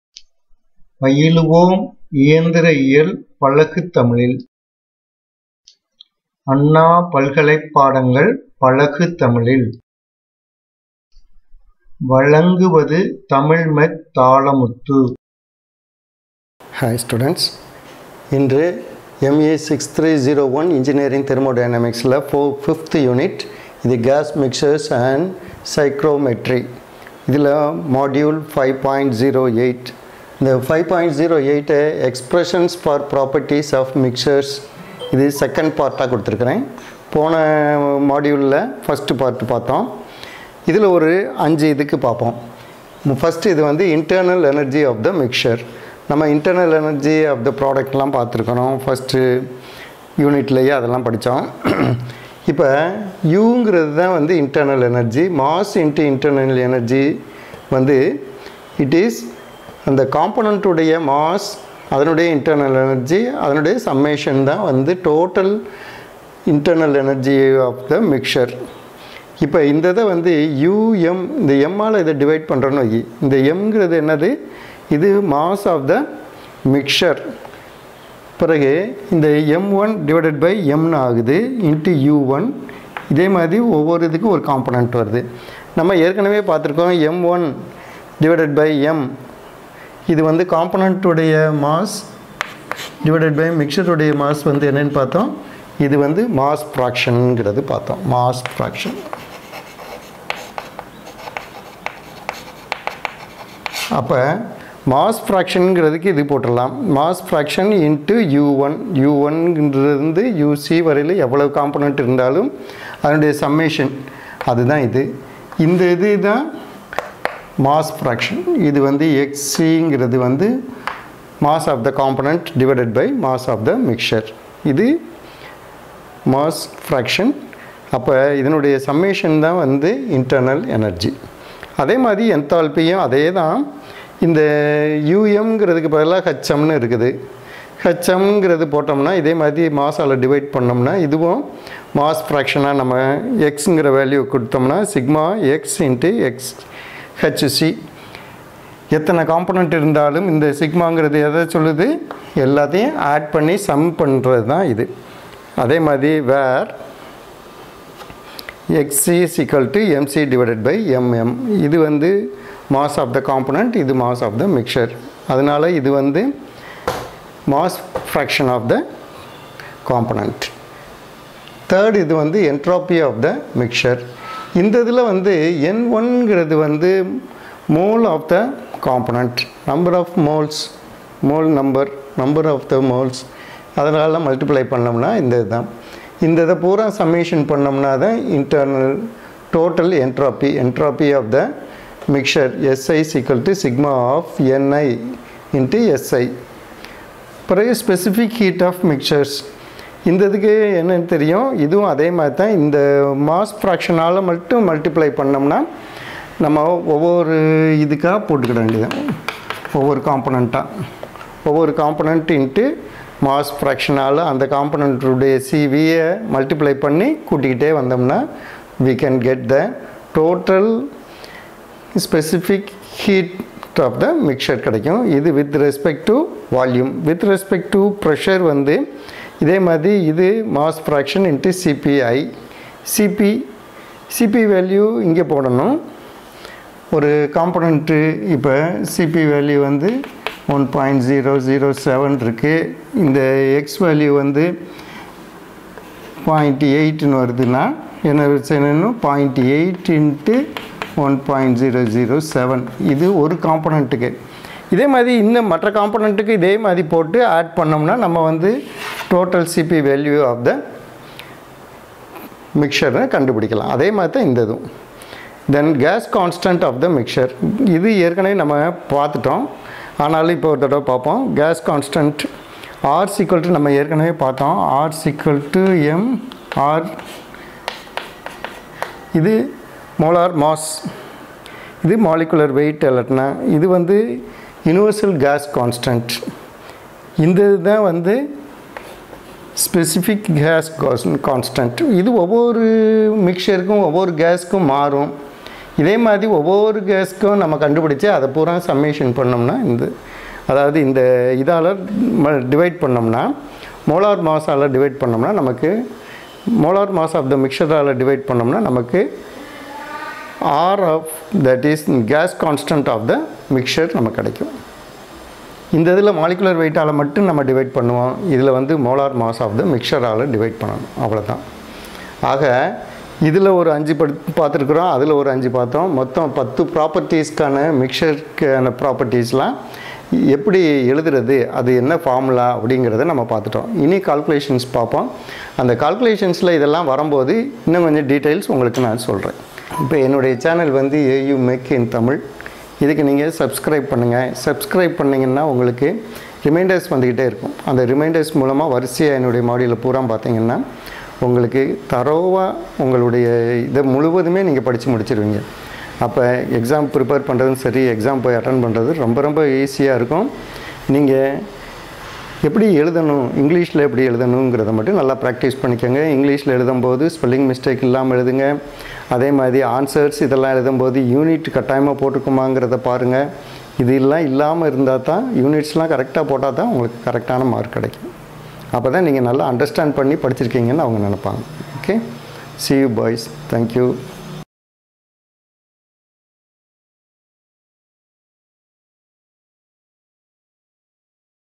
Expressions for Properties of Gas Mixtures - M5.08 - Enigneering Thermodynamics in Tamil
I hereby explain various expressions for properties of gas mixtures in Tamil.